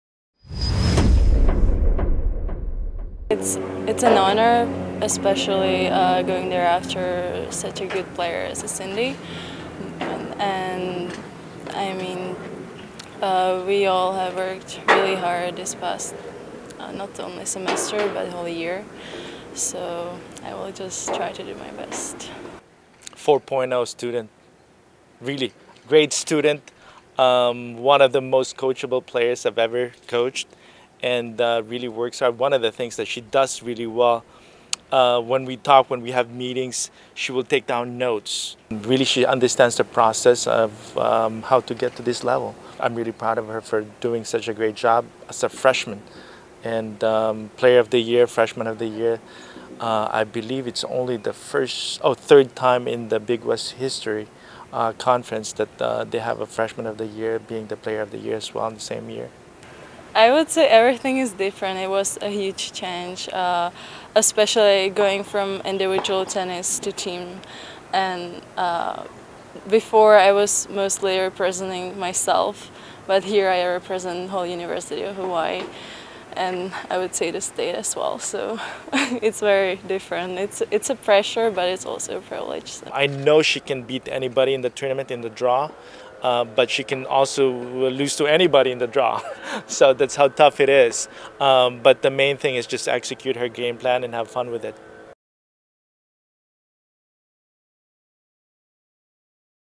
Petra Melounova Competes in NCAA Women's Singles Tournament
University of Hawai‘i freshman Petra Melounova will head to Athens, Ga. to compete in the 2017 NCAA DI Women's Tennis Championships. Hear thoughts from Petra and head coach Jun Hernandez